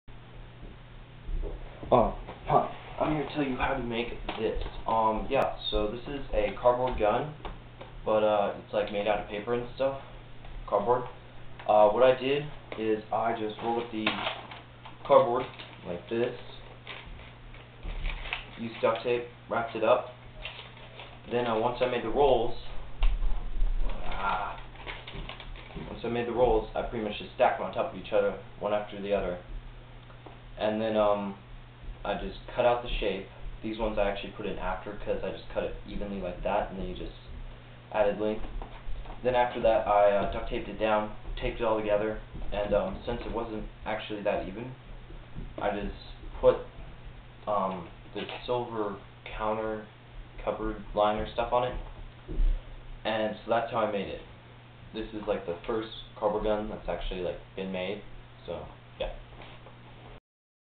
halo gun paper gun !
I made the 1st cardboard gun and im going to tell you how to now